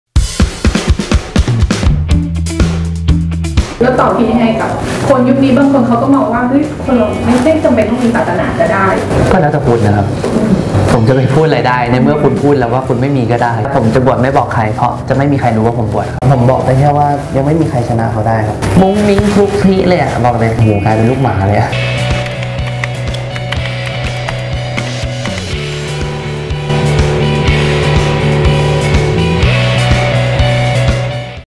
ต่อ - ธนภพ ลีรัตนขจร (PRAEW Talk : TEASER)
หลังจากกลายเป็นพระเอกมือรางวัล ชีวิตของเขานับจากนี้ มีอะไรที่เปลี่ยนไปบ้าง พบกับ ต่อ-ธนภพ ลีรัตนขจร ที่แพรวทอล์คได้เร็วๆ นี้